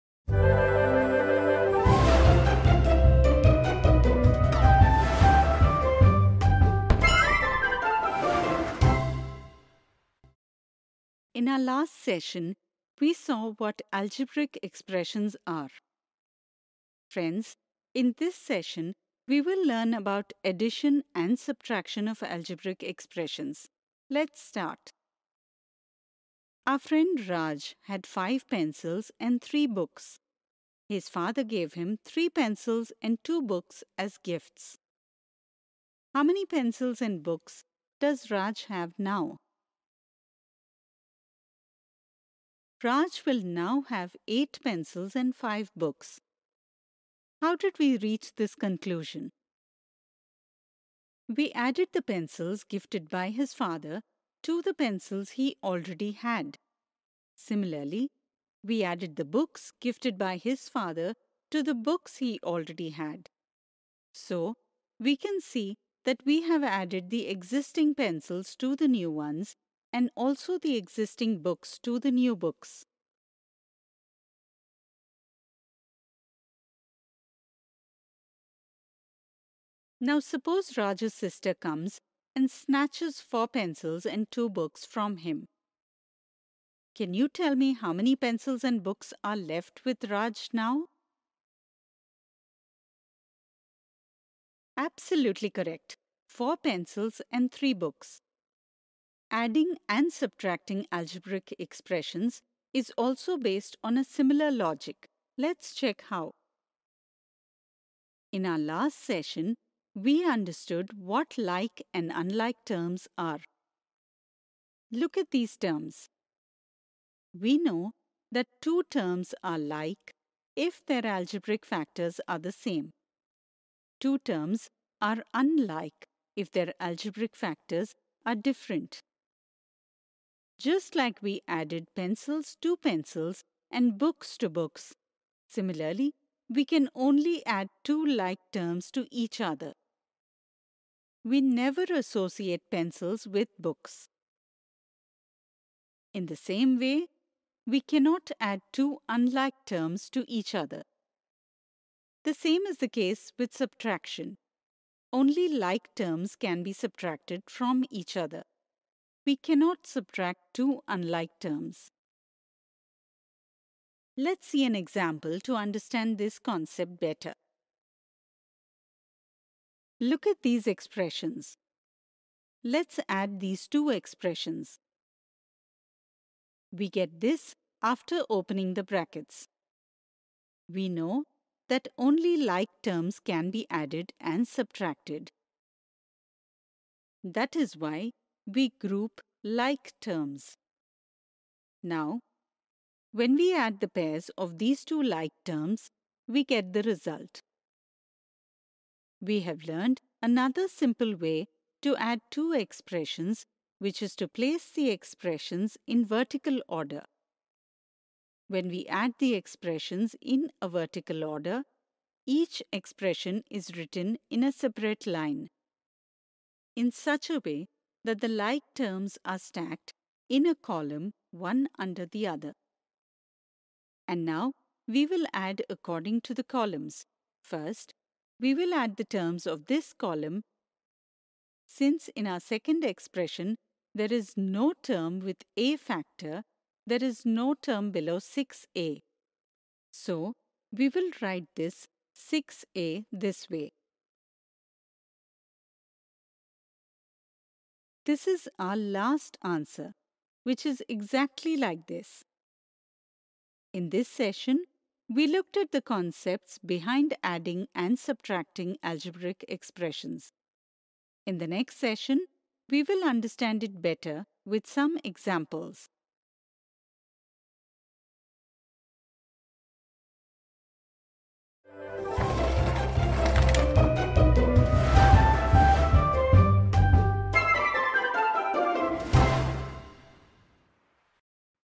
Addition and subtraction of algebraic expressions | Part1/3 | English | Class 8
Addition and subtraction of algebraic expressions | Part1/3 | English | Class 8 | NCERT

Addition and Subtraction of Algebraic Expressions | Algebra | Grade 8 | NCERT | Algebraic expressions and identities

Welcome to part 1 of Addition and Subtraction of Algebraic Expressions. In this video, we will discuss the concept of Addition and Subtraction of Algebraic Expressions.